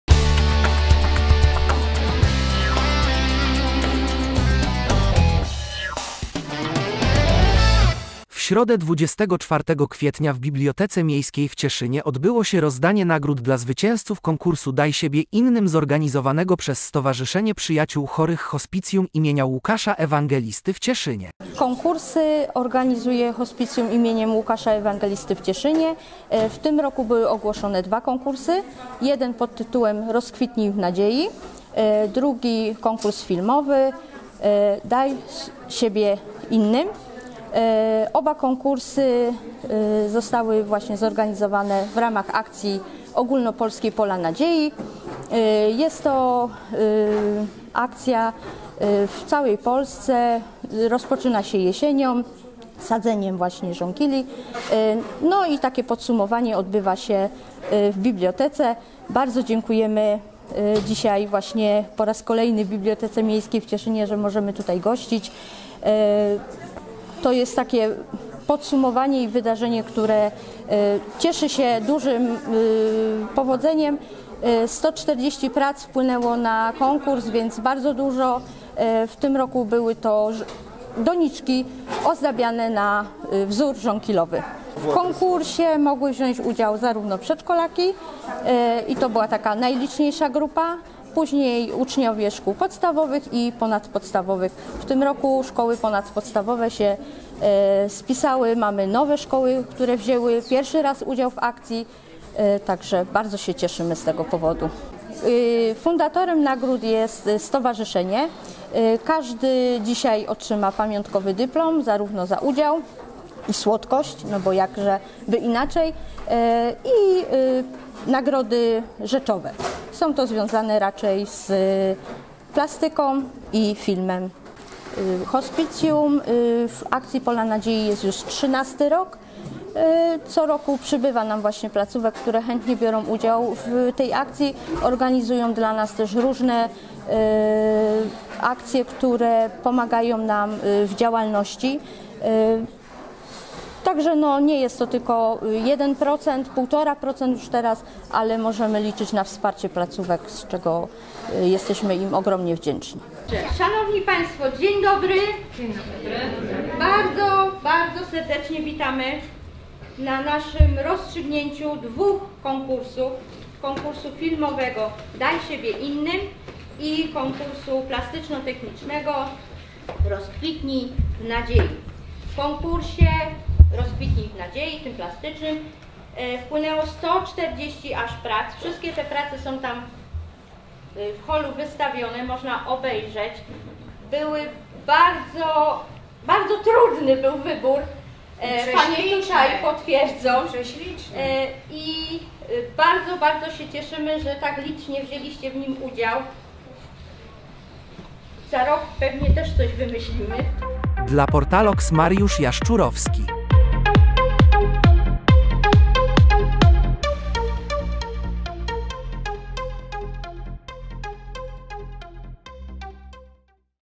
Rozdanie nagród dla zwycięzców konkursów "Daj siebie innym"i "Rozkwitnij w nadziei"
Cieszyn 24.04.24
Rozdanie nagród dla zwycięzców konkursów "Daj siebie innym"i "Rozkwitnij w nadziei"  zorganizowanego przez Stowarzyszenie Przyjaciół Chorych Hospicjum imienia
Łukasza Ewangelisty w Cieszynie